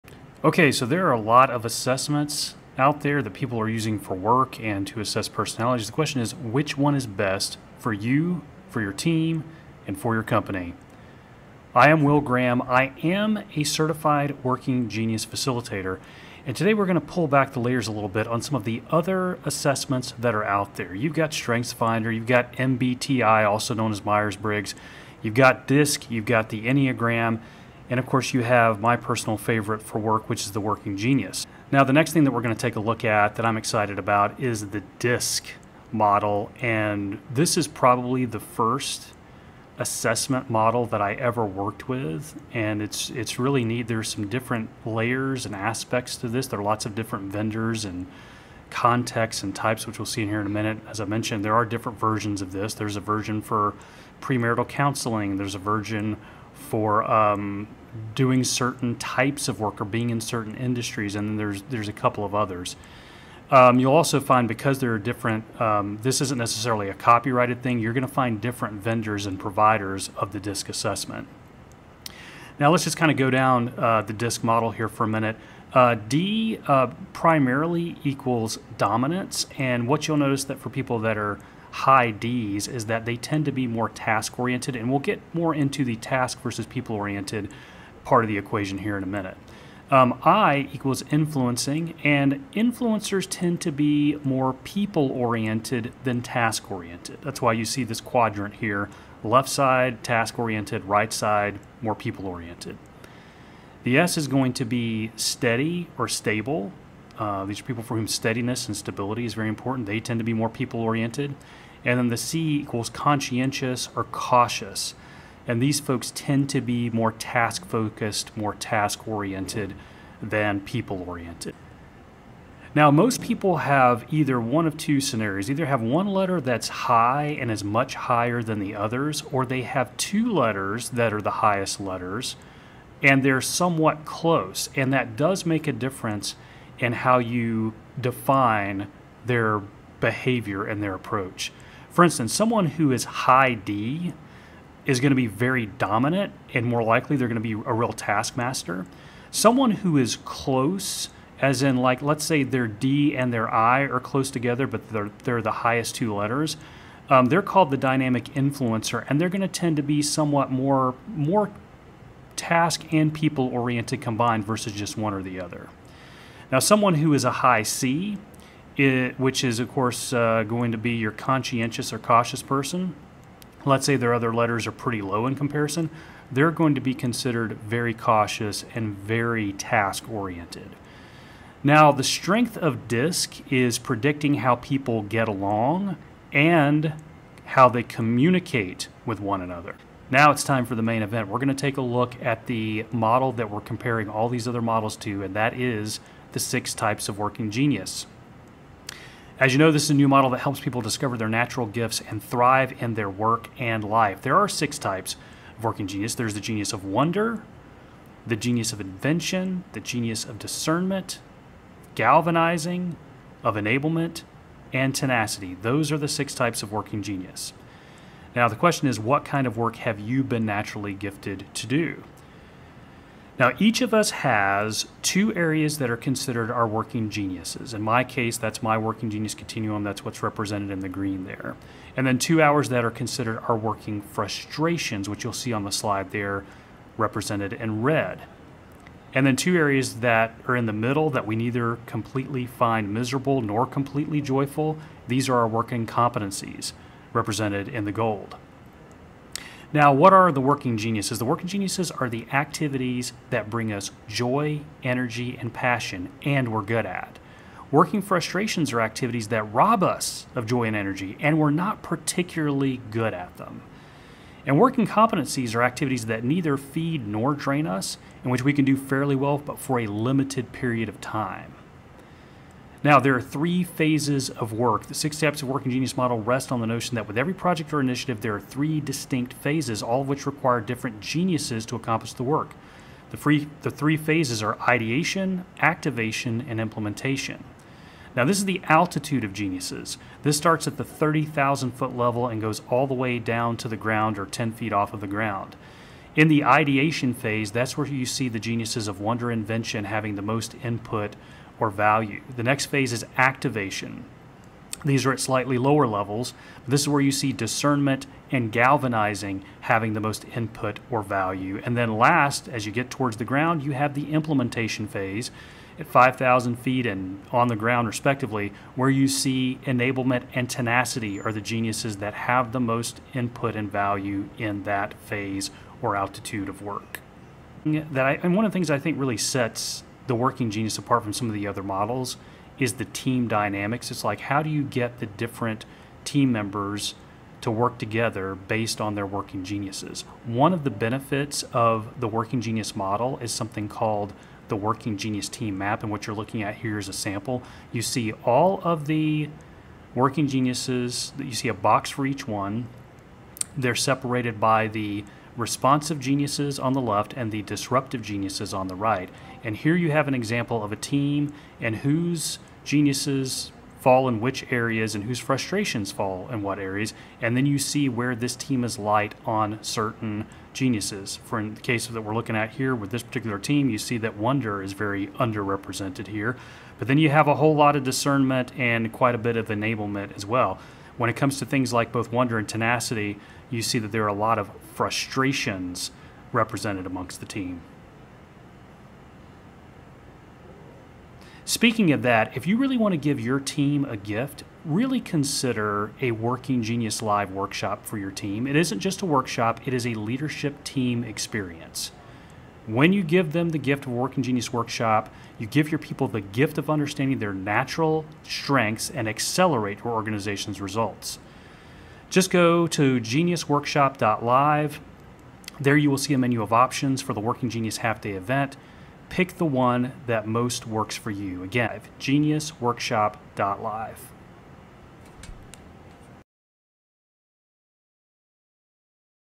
Team Assessment Model Comparison - Working Genius vs DISC
How does Working Genius compare to the other major Personality Assessments out there? Today we'll go 6 Types of Working Genius vs. the DISC model.

For more information on a Working Genius Workshop for your team,

To download 2 sample chapters of my book: Make Work Better, How Empowered Teams Transform Work and Deliver Better Results, go